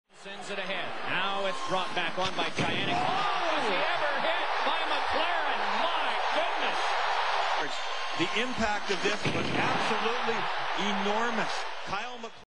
Kyle McLaren levels Cajanek
This is a monster hit from the Sharks vs Blues playoff series a few years ago.